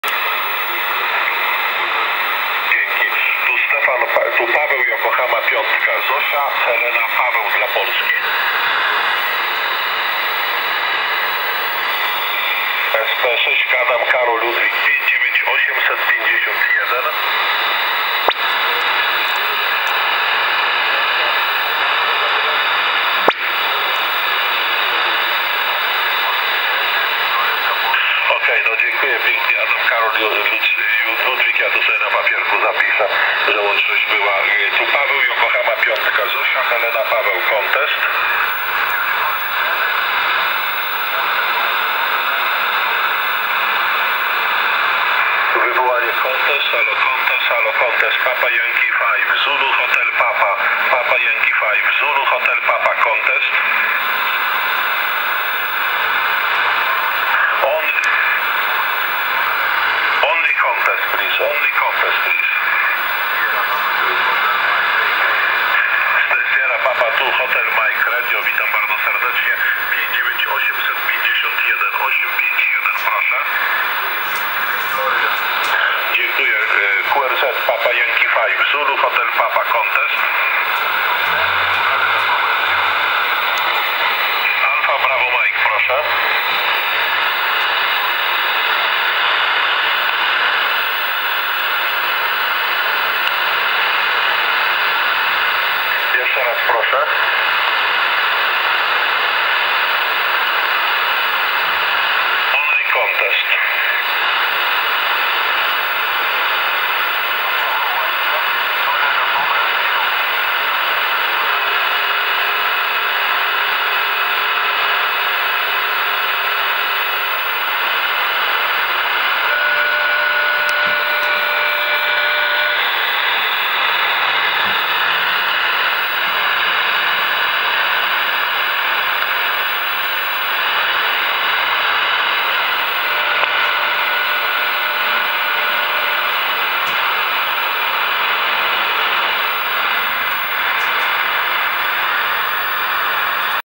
SP DX Contest 2014 PY5ZHP 10m SSB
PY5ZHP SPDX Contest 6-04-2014
RX: Intek Galaxy Uranus
ant: homebrew vertical